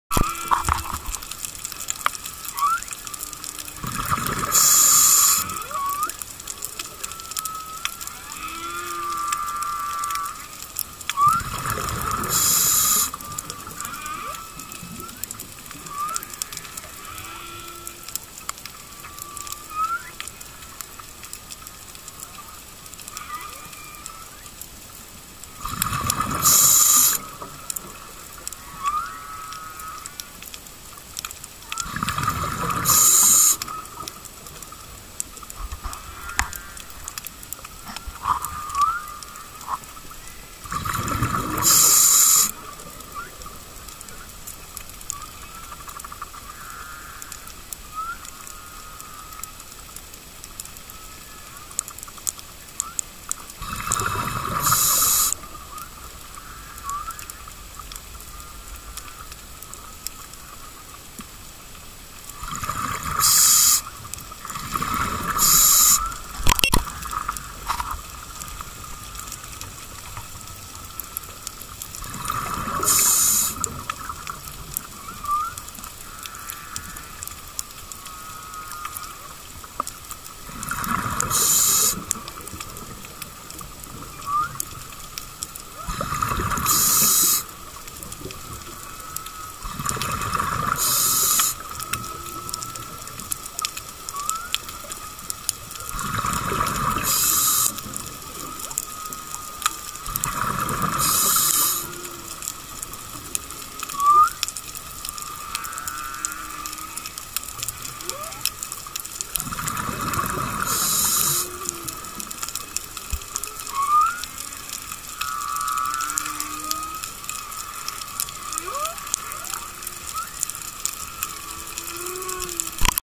Goatfish School, Bigeye Scads, and Whales Singing
Reef at Makena Landing, Maui.